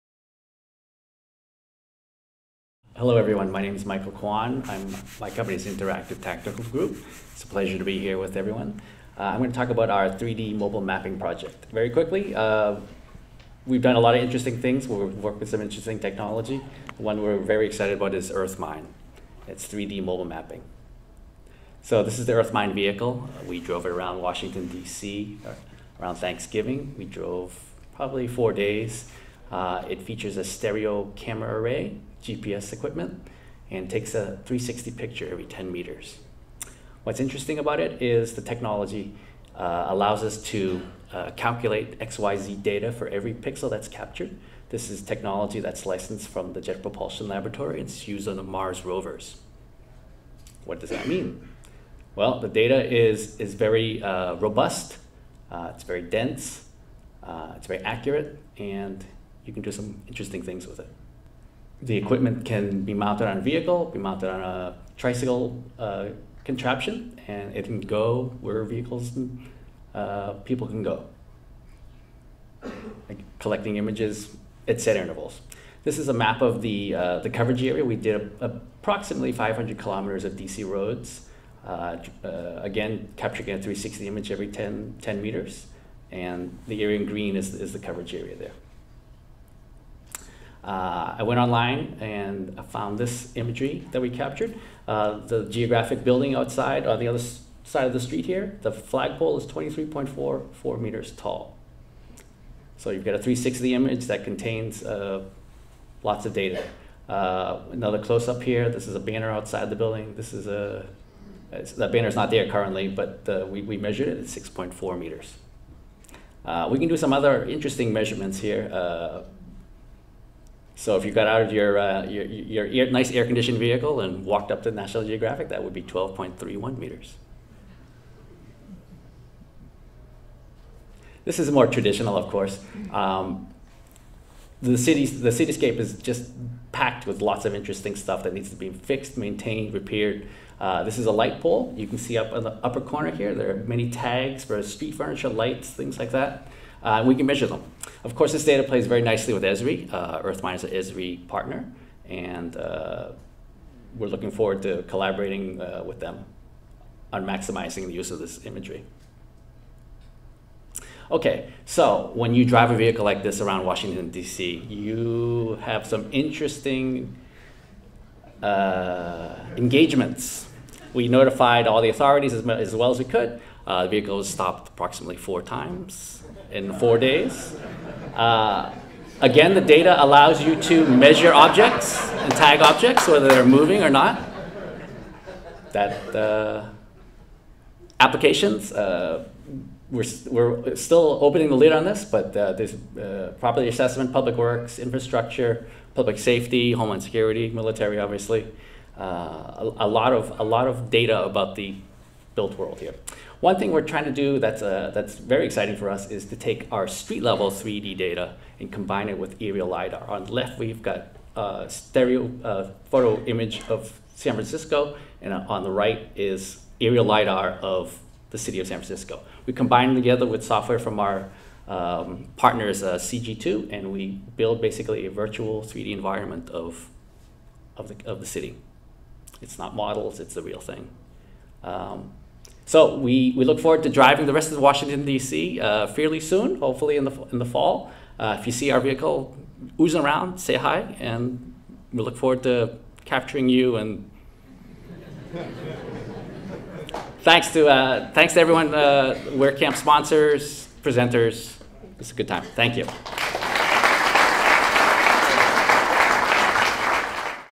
3D Mobile Mapping Project of Washington DC at WhereCampDC
Michael Quan of Interactive Technical Group: "3D Mobile Mapping Project of Washington DC" at WhereCampDC's Friday June 10th Spatial Ignite.

Video by TuckerWalsh.com